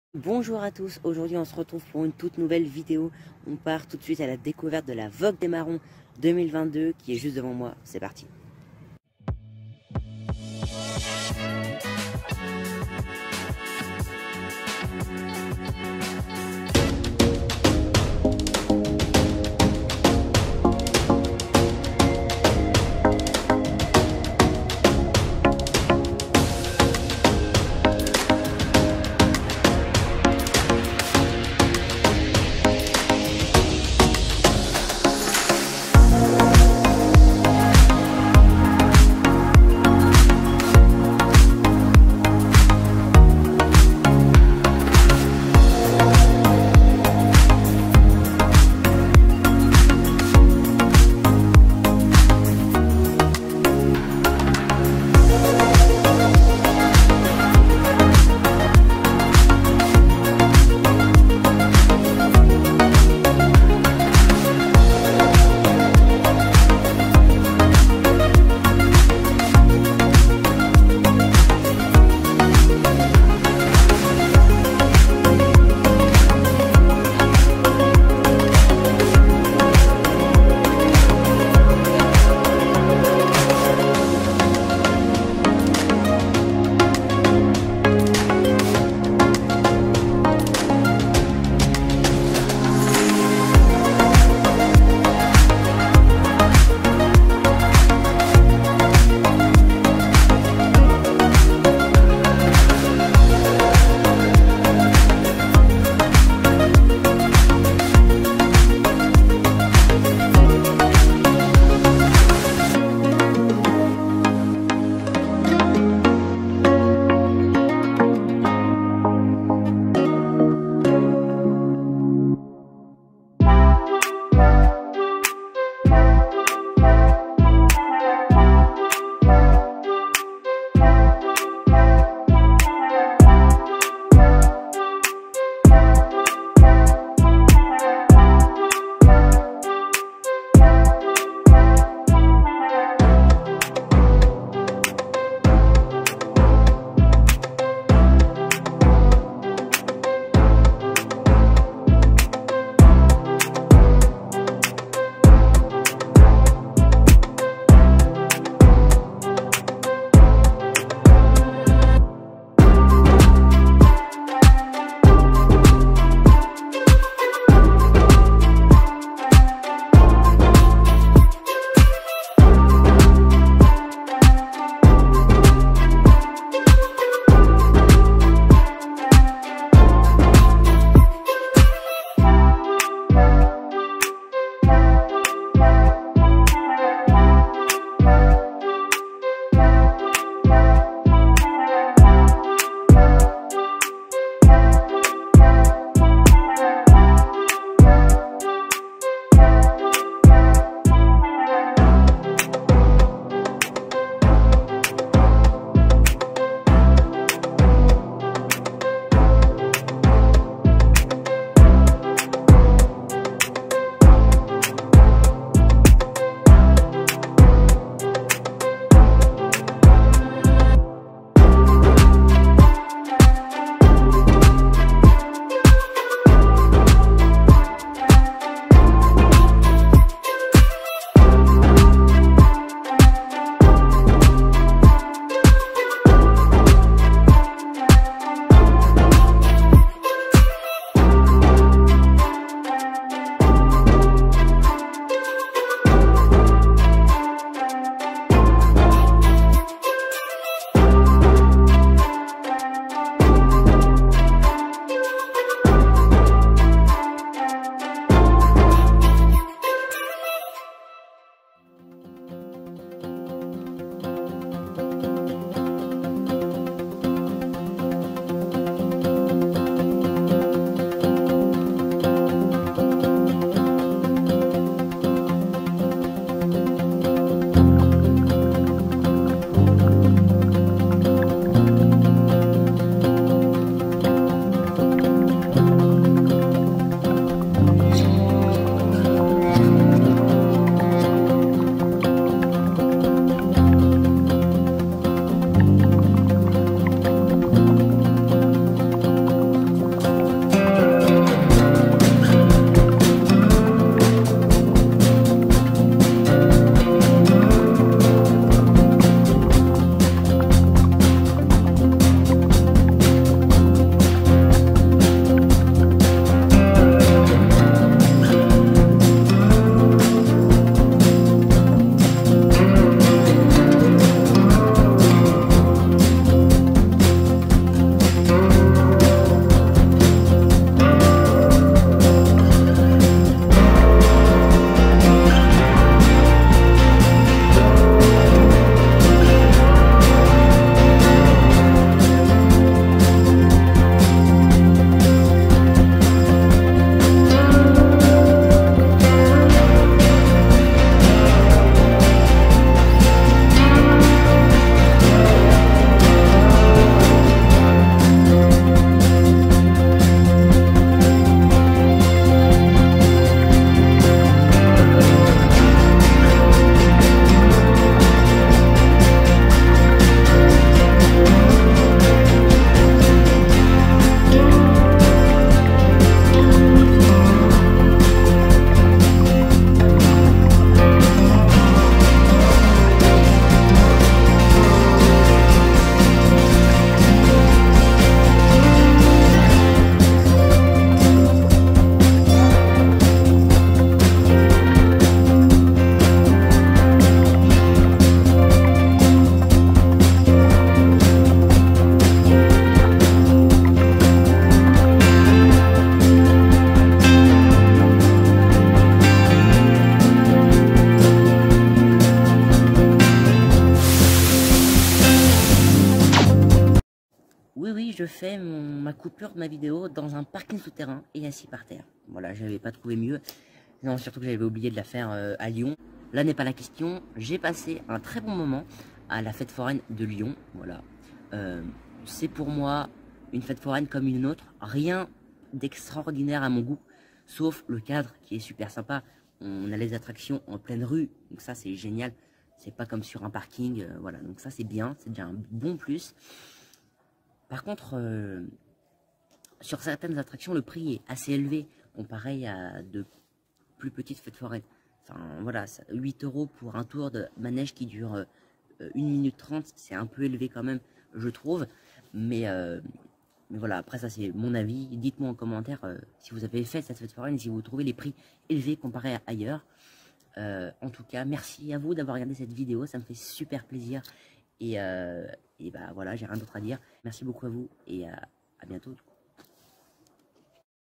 Vlog {} Fête Foraine Lyon (2022)
Vogue des Marrons 2022

Bonjour à tous et bienvenue sur cette toute nouvelle vidéo pour se plonger dans la Vogue des Marrons 2022